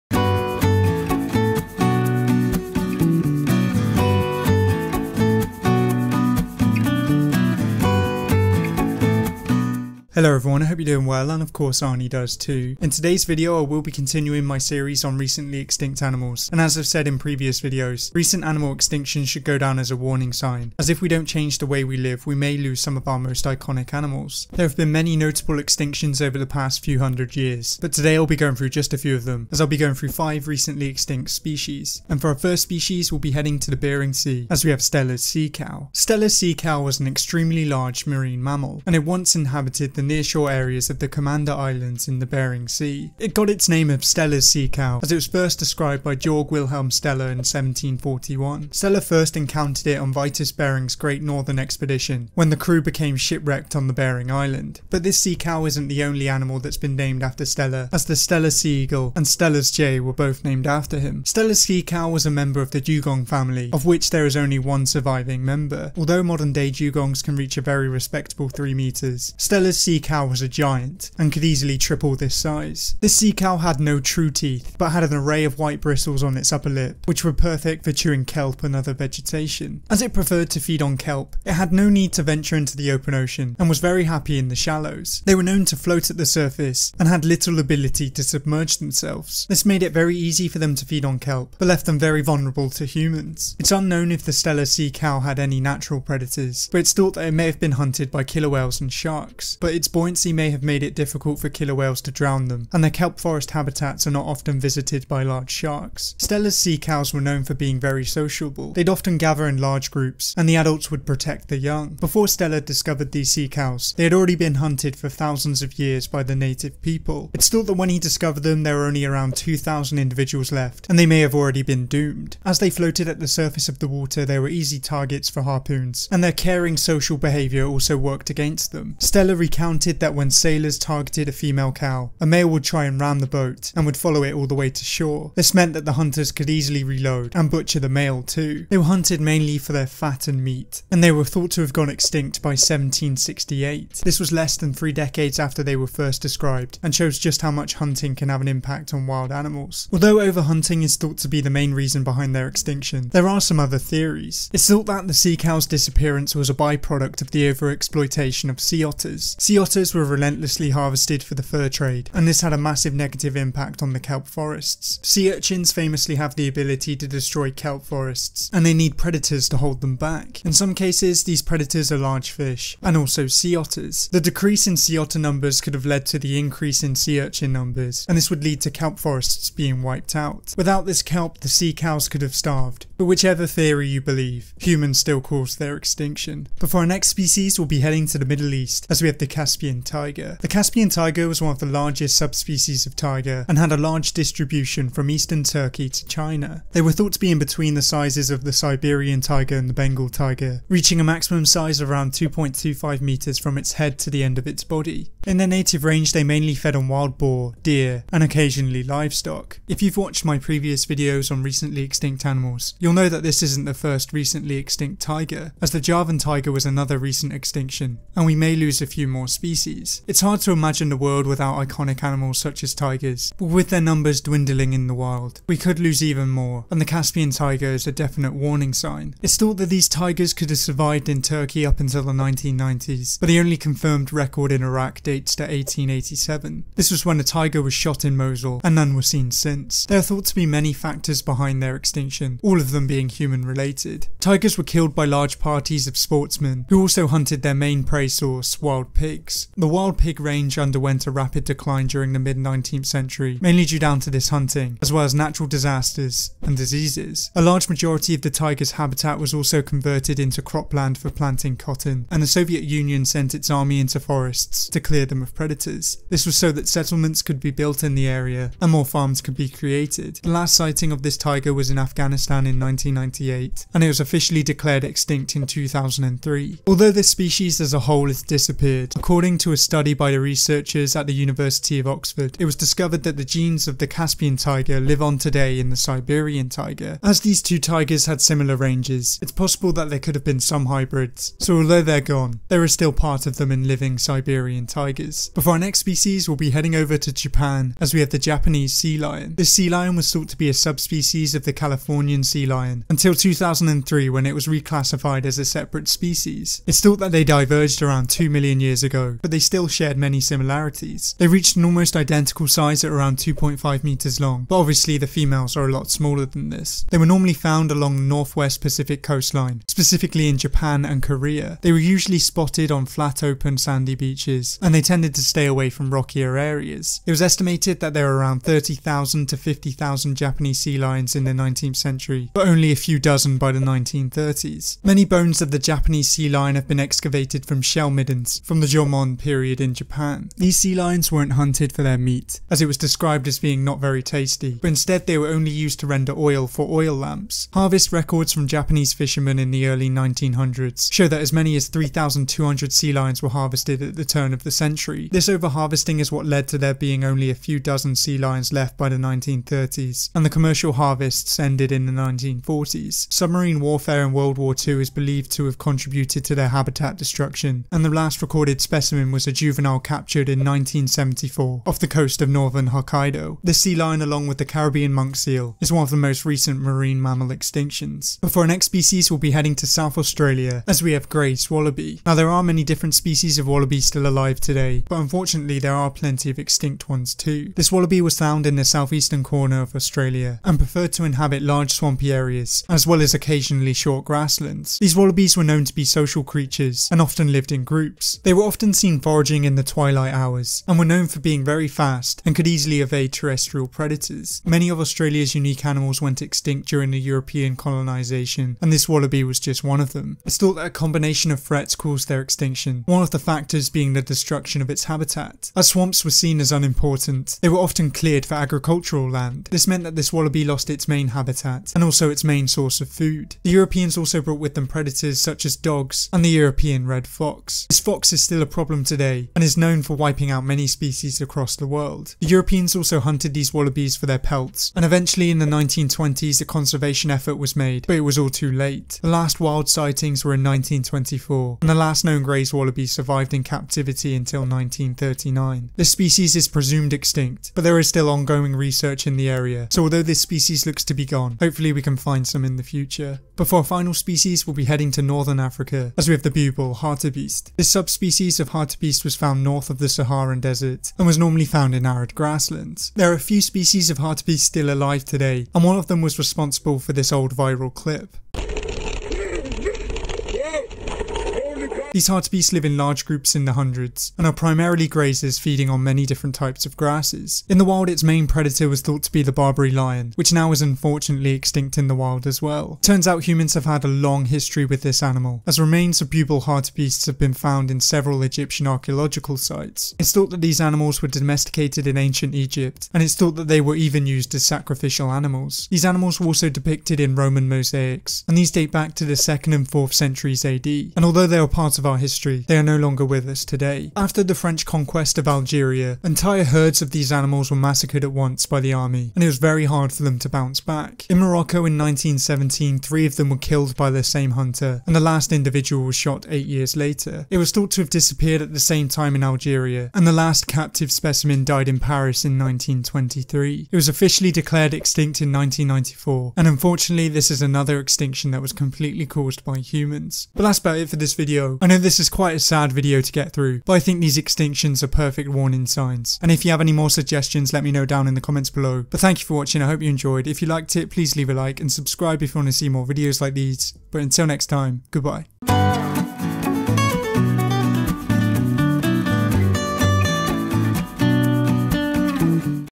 5 Recently Extinct Animals Gone But Not Forgotten
Over the past few hundred years there have been many famous animal extinctions. Most recent animal extinctions are all down to humans with global warming and habitat destruction being the main factors. In this video I will be going through just a few of these recent animal extinctions as I will be going through 5 recently extinct animals gone but not forgotten.

Attributions

Stellar's sea cow images:
KKPCW
F. John
Fedor S. Sharko et al.

Japanese sea lion images:
Togabi
Kawahara Keiga
Pagemoral
Contributor Pagemoral takes a photograph

Vitus Bering image:
Институт Археологии РАН

Dugong footage:
Pro.snorkeling

California sea lion image:
Franco Folini

Shell midden image:
Namazu-tron

Barbary lion image:
Michael J. Litzinger

Hartebeest image:
. Ray in Manila

For our first recently extinct species we will be heading to the Bering sea as we have the Steller's sea cow. Steller's sea cow was a large member of the dugong family and was thought to have gone extinct due to overhunting.

For our next recently extinct species we will be heading to the middle east as we have the Caspian tiger. The Caspian tiger was one of the largest subspecies of tiger and roamed from eastern turkey to China. The Caspian tiger was thought to have gone extinct due to overhunting and overhunting of their prey.

Foe our next recently extinct species we will be heading to the coastal waters of japan as we have the Japanese sealion. The Japanese sea lion was very similar in size and body shape to the closely related California sea lion. The Japanese sea lion was hunted mainly for oil and went extinct in the early 1900s.

Foe our next recently extinct animal we we be heading over to southern Australia as we have Grey's Wallaby. Grey's wallaby was a swamp loving wallaby and was known for being sociable and nocturnal. Grey's wallaby went extinct after European colonisation but there still may be some out there.

For our final recently extinct species we will be heading to north Africa as we have the Bubal hartebeest. The bubal hartebeest has a long history with humans and was domesticated in ancient Egypt. It's thought that these creatures went extinct due to overhunting and although they're gone they will not be forgotten.